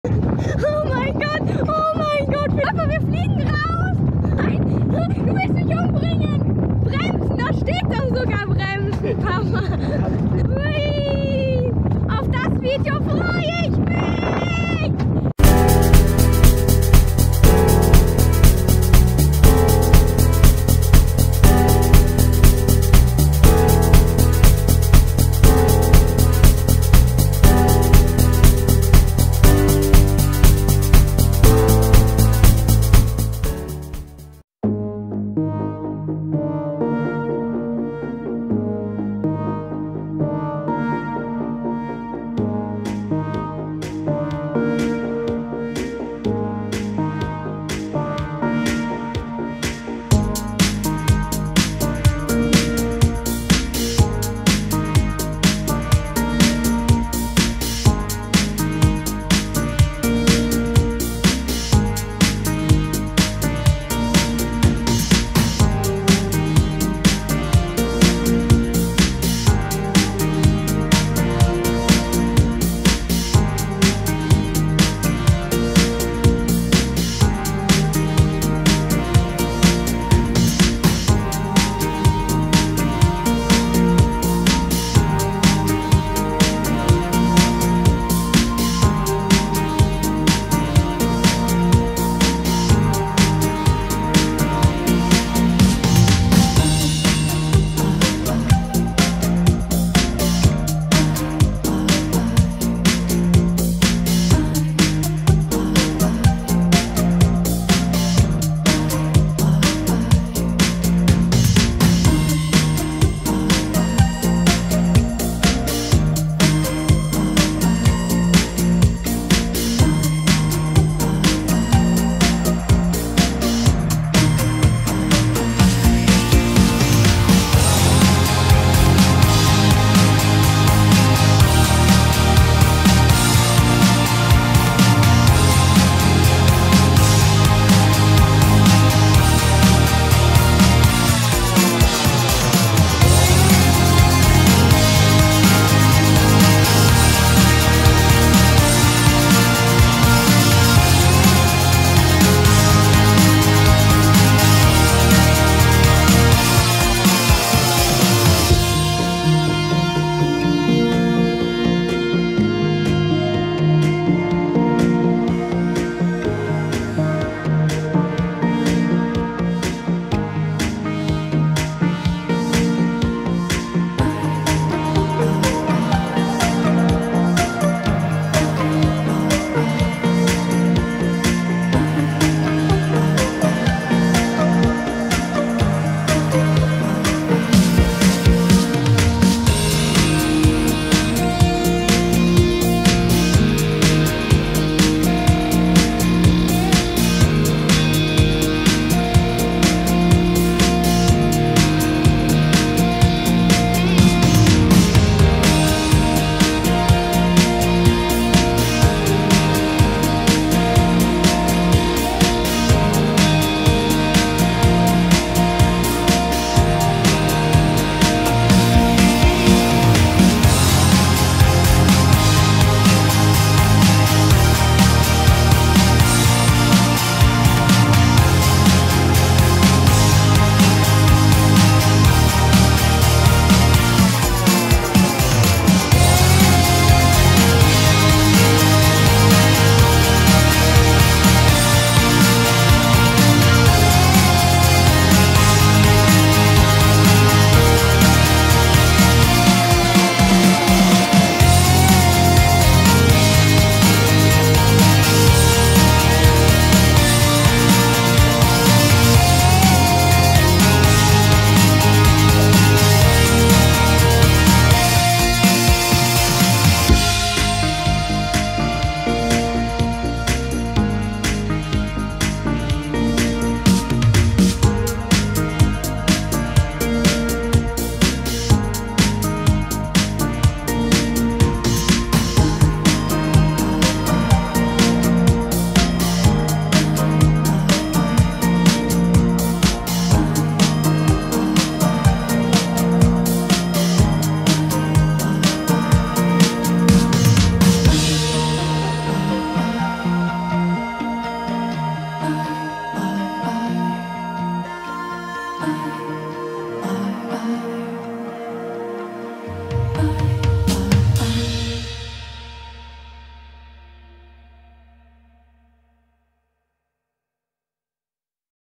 Alpin Coaster Golm Montafon 2022
Eine ca. 5 minütige Fahrt mit dem Allwetter Alpin Coaster in Golm (Vorarlberg / Montafon) - Fast so schön wie am Moped...nur sicher für meinen Sohn und mich.
2600 Meter mit 44 aufregende Jumps, spektakulärer 360°-Kreisel und 15 wilde 180°-Kehren. Neigung von bis zu 40° und bis zu 6 m über dem Boden.